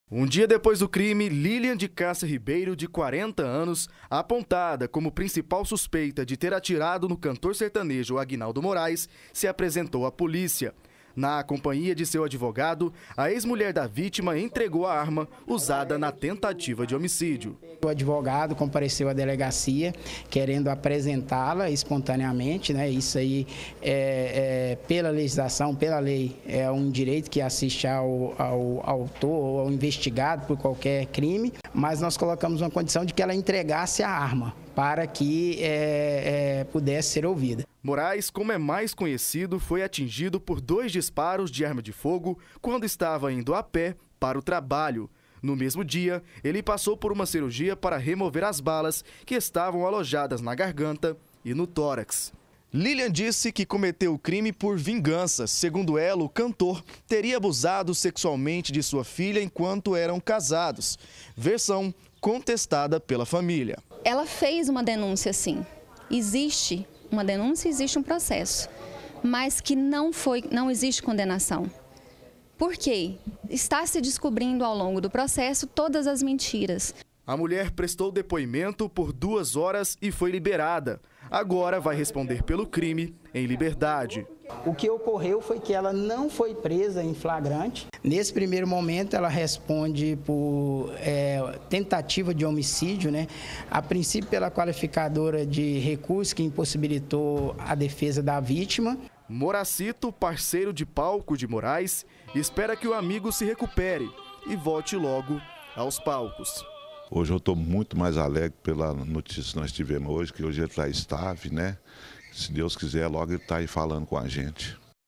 MULHER SUSPEITA DE ATIRAR EM CANTOR SERTANEJO SE APRESENTA À POLÍCIA
A mulher suspeita de atirar no ex-marido, o cantor sertanejo Agnaldo Morais, se apresentou à polícia. Ela confessou o crime.